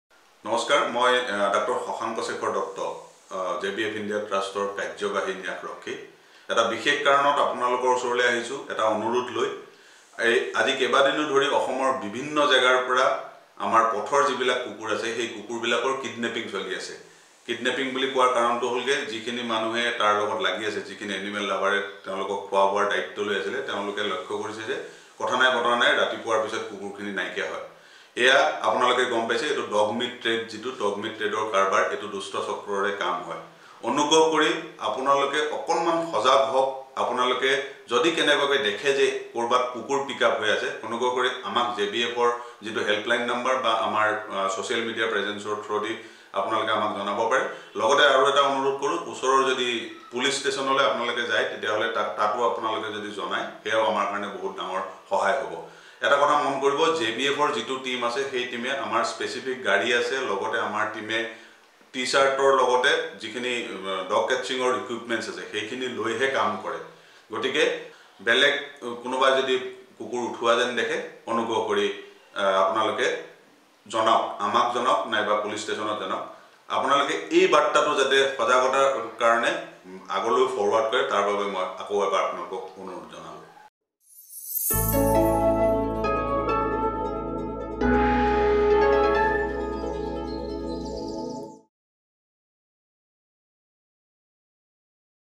Illegal abduction of dogs in Assam
Dog meat trade is one of the most serious animal welfare issue in Asia. Many animal welfare organizations across the continent have been fighting against it for many years now.
Like them JBF (INDIA) TRUST too have been actively working to stop such a heinous activity in Assam.
This video is intended to inform the audiences about the recent frequent abduction of dogs illegally in Assam.